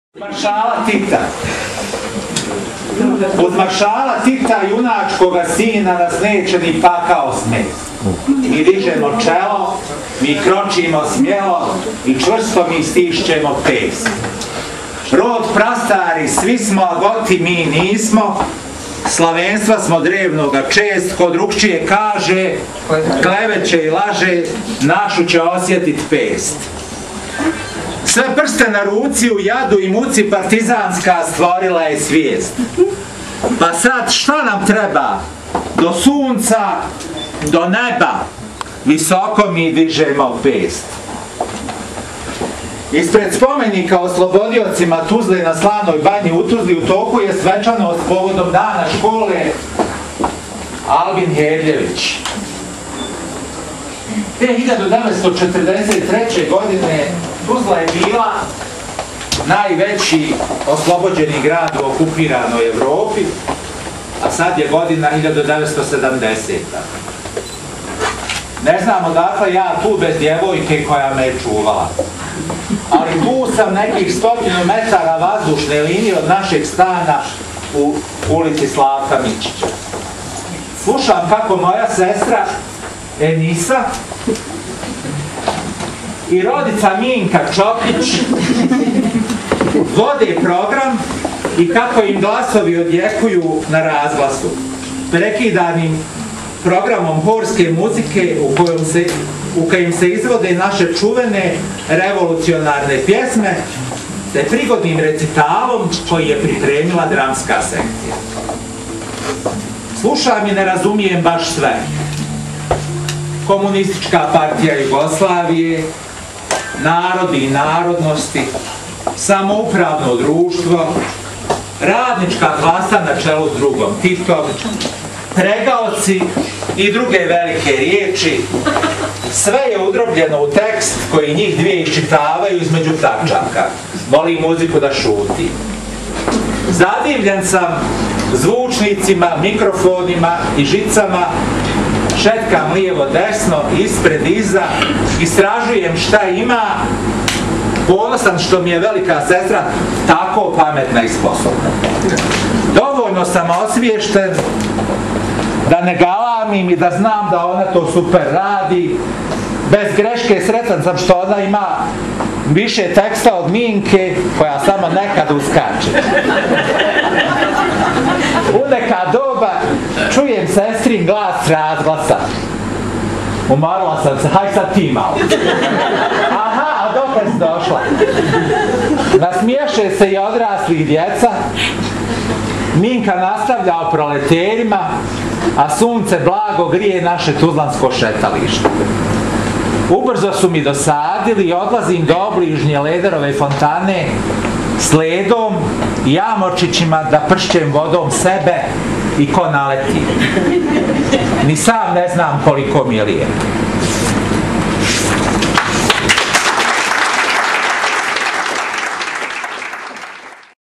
Predstavljanje knjige 'Četveropis vremena' 09/13
Predstavljanje knjige 'Četveropis vremena' u Američkom kutku u Nacionalnoj i Univerzitetskoj biblioteci 'Derviš Sušić' u Tuzli. Autori knjige: Samija, Džemal, Enisa i Nihad Mešić. Izdavač knjige je 'Paun' Tuzla. Promotorice knjige: Alma Omeragić i Majda Zukić Ribić. Muzički program: Edin Maglić. Moderatorica promocije: Sanja Dukić.